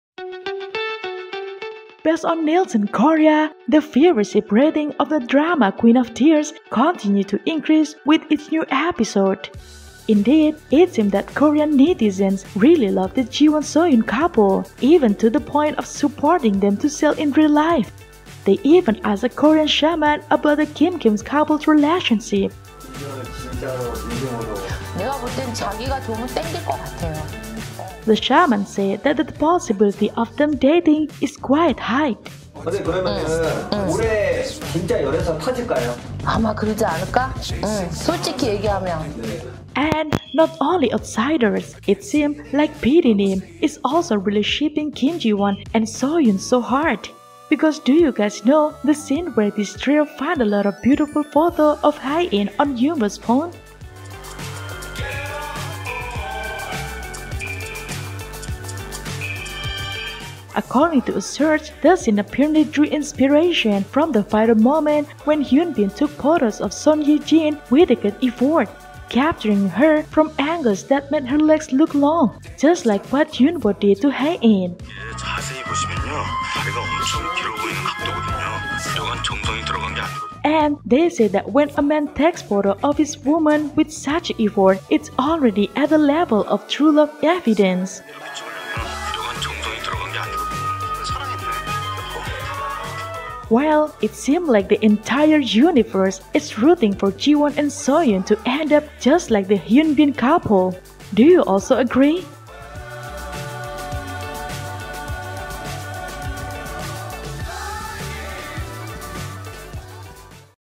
Soohyun & Jiwon must know that the whole universe approves of them being together😭👏Even PD-nim!!?
Based on Nielsen Korea, the viewership ratings of the drama "Queen Of Tears" continue to increase with each new episode. Indeed, it seems that Korean Netizens really love the Jiwon Soohyun couple, even to the point of supporting them to sail in real life.

They even asked a Korean shaman about the KimKim couple's relationship. The shaman said that the possibility of them dating is quite high.

And not only outsiders, it seems like PD-nim is also really shipping Kim Jiwon and Soohyun so hard. Because do you guys know the scene where this trio finds a lot of beautiful photos of Haein on Hyunwoo's phone?

According to a source, that scene apparently drew inspiration from the viral moment when Hyun Bin took photos of Son Ye Jin with great effort, capturing her from angles that made her legs look long,  just like what Hyunwoo did to Haein.

And they say that when a man takes photos of his woman with such effort, it's already at the level of true love evidence.

Well, it seems like the entire universe is rooting for Jiwon and Soohyun to end up just like the Hyunbin couple! Do you also agree guys..?